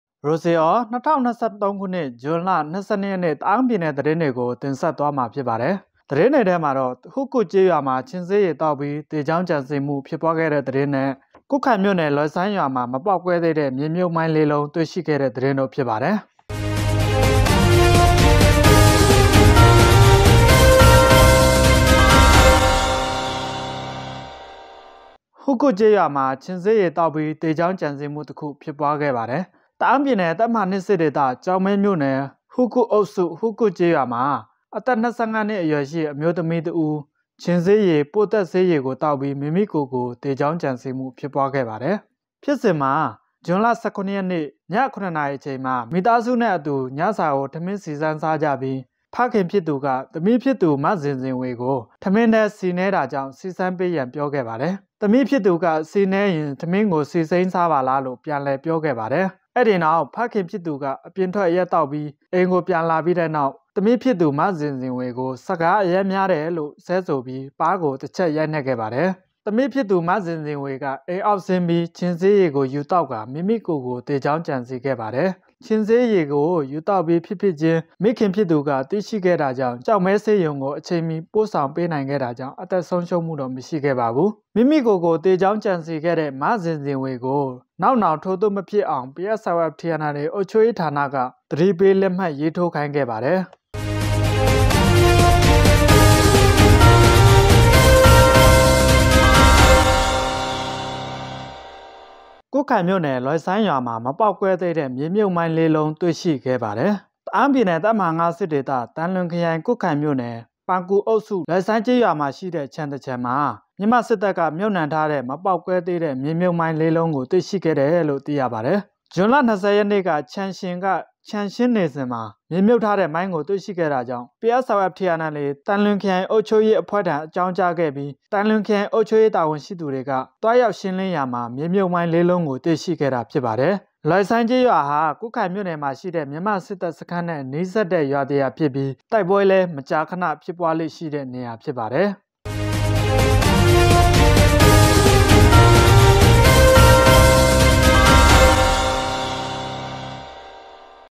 Ta'ang State News ဗမာဘာသာ 22.6.2023
1. ဟူးကွတ်ကျေးရွာဒေသခံတစ်ဦး ခြင်ဆေးရည်သောက်ပြီး သေကြောင်းကြံစည်းမှု ဖြစ်ပွား

2. ကွတ်ခိုင်မြို့နယ်၊ လွယ်ဆိုင်းရွာတွင် မပေါက်ကွဲသေးသည့်မြေမြုပ်မိုင်း(၄)လုံး တွေ့ရှိ

Ta'ang State News ဗမာဘာသာ